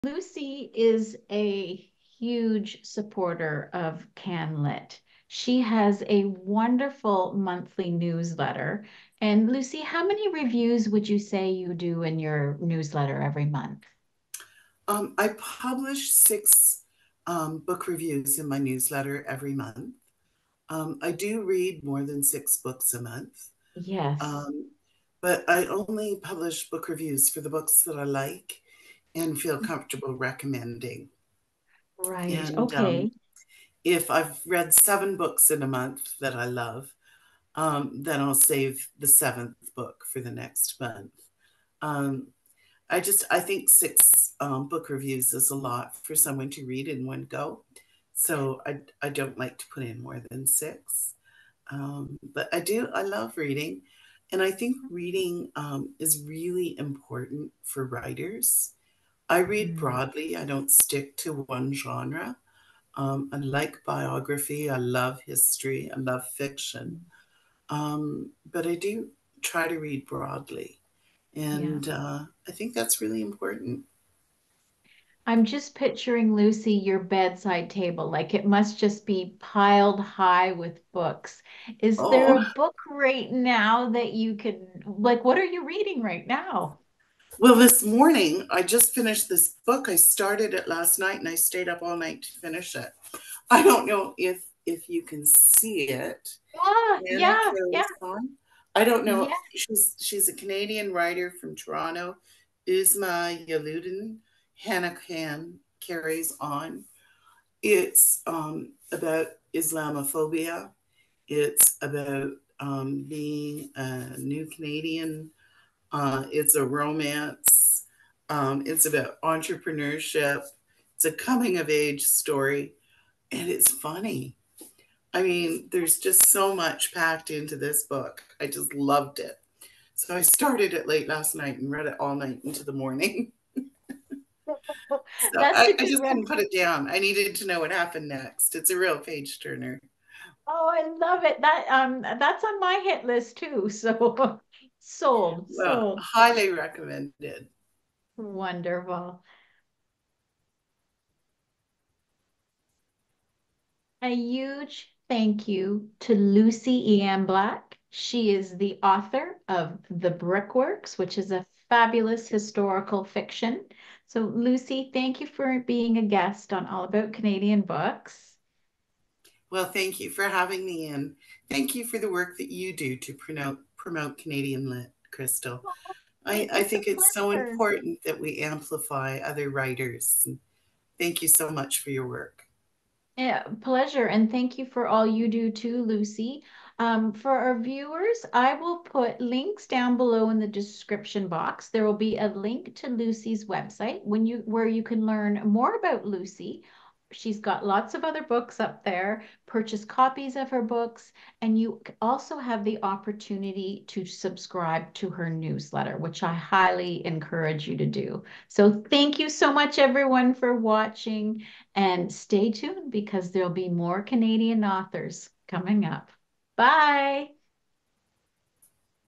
What's Author Lucy EM Black Reading?
In this segment, author Lucy EM Black tells us what book is on her bedside table.

Lucy E.M. Black, is the author of The Marzipan Fruit Basket, Eleanor Courtown, Stella’s Carpet, and The Brickworks.  Her short stories have been published in Britain, Ireland, USA and Canada in literary journals and magazines.  She lives in Port Perry, Ontario, the traditional territory of the Mississaugas of Scugog Island, First Nations.

AACB  is situated on the traditional territory of the Anishinaabeg people.

Uzma Jalaluddin’s Hana Khan Carries